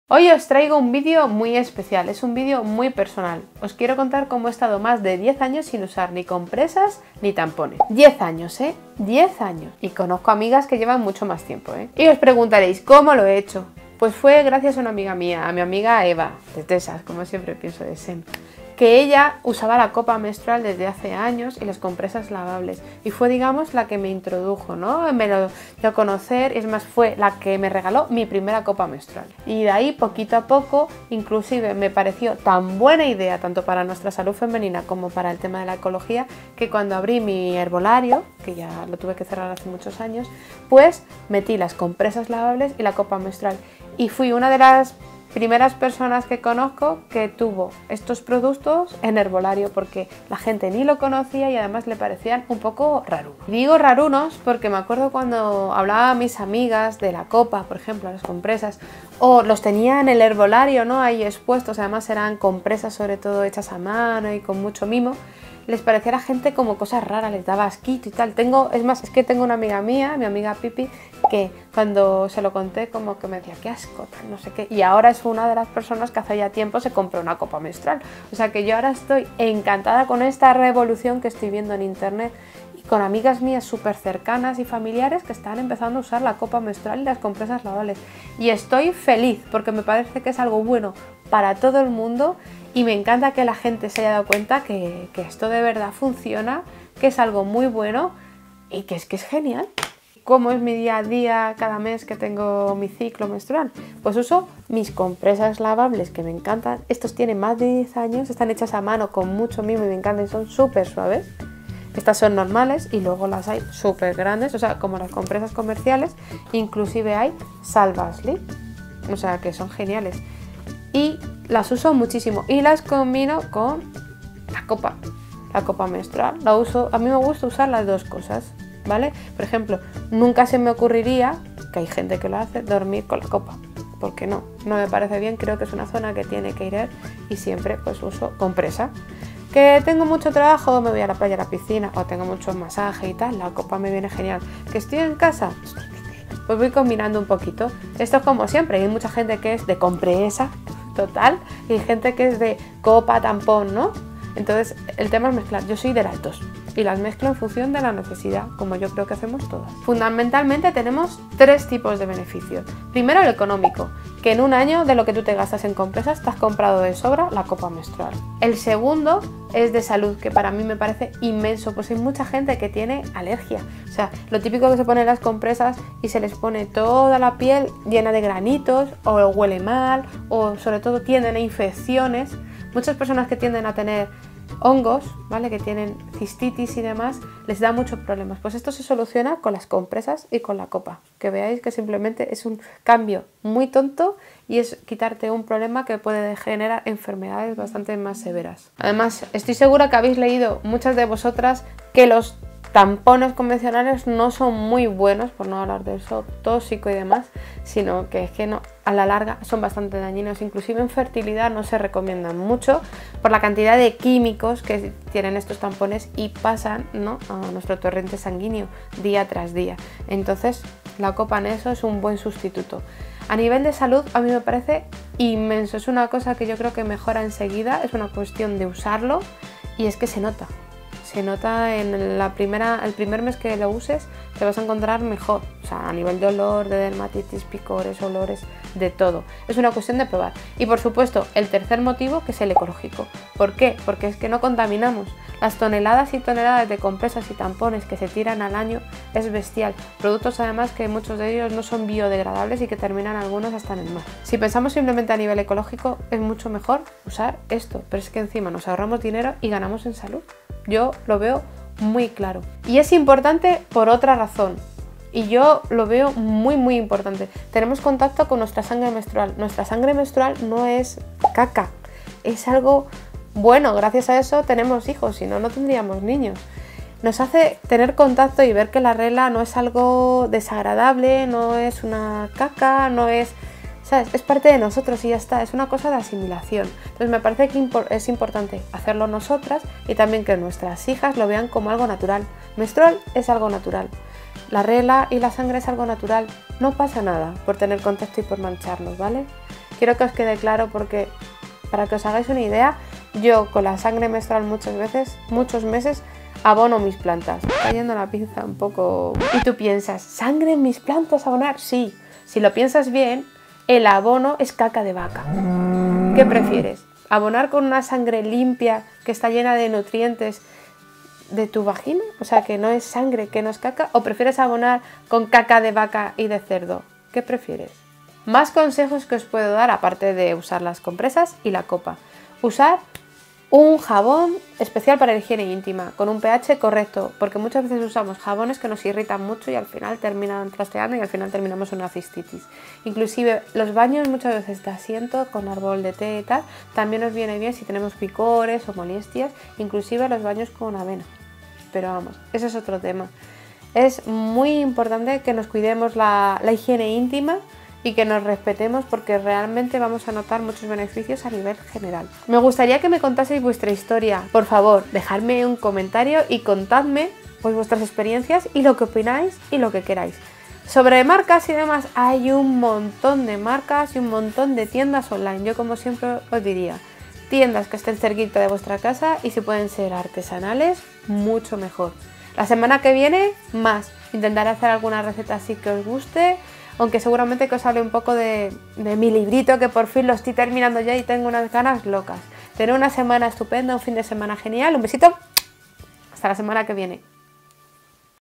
CONSEJOS PARA UNA MENSTRUACIÓN SANA
Llevo mucho tiempo con ganas de hablaros sobre las opciones sanas y eco que podemos usar durante nuestra menstruación.

Yo llevo usando compresas lavables y la copa menstrual, más de diez años y estoy encantada. Por eso, quiero compartirlo con vosotros.

Con este vídeo espero animaros a que probéis estas alternativas saludables, estoy segura que os sorprenderán :-)

Por favor, compartid vuestras experiencias y opiniones en el canal. Así aprendemos todos :-)